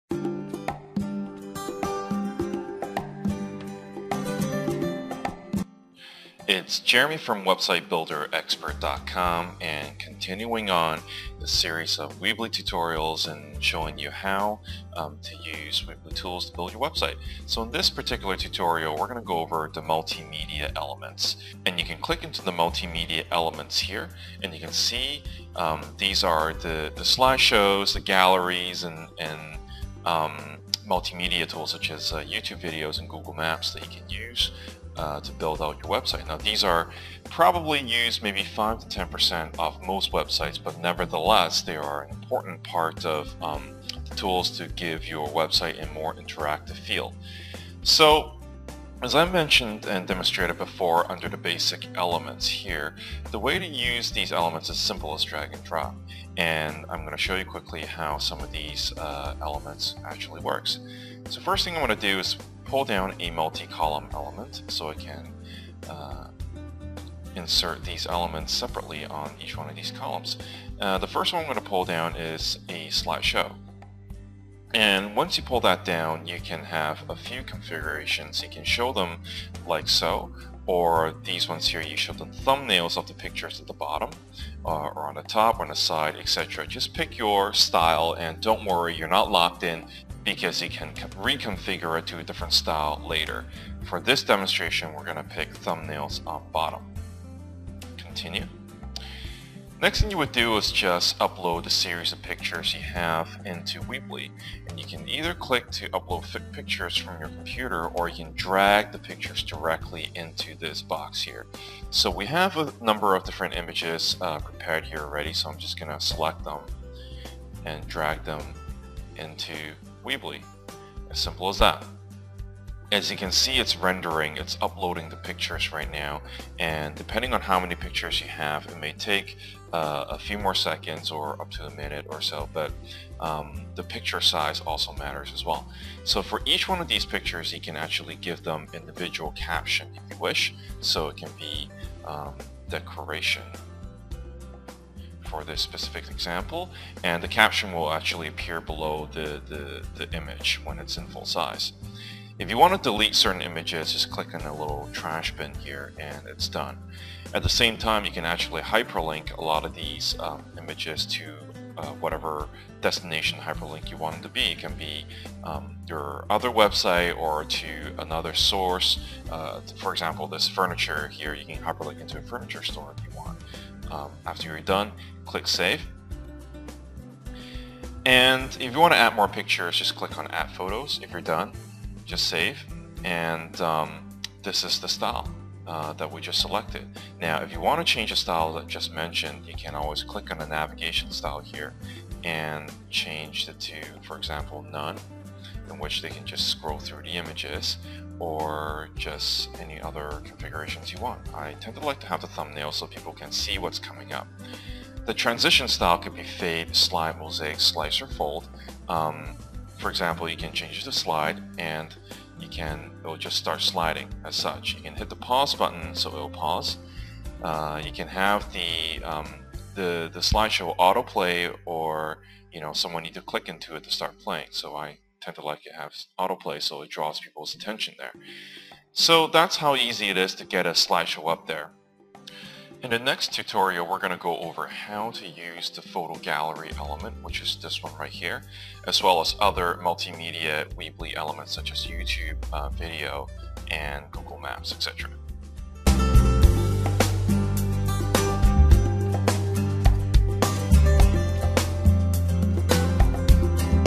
How to Use Weebly Slideshow Element (by
The Weebly Slideshow Element is simple to use, and this video will take you step-by-step how to set up the element.

Using this Weebly multimedia element is one of the fastest way to make your website more interactive and to improve its design.

To get a good understanding of the pros and cons of using Weebly to build your for an expert opinion!